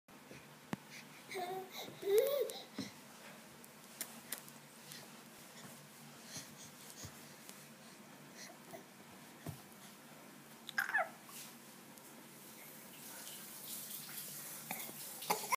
Sonia's first crawl
We did not knew she already learnt it. She has started standing one month back and on 23 sept she suddenly started crawling.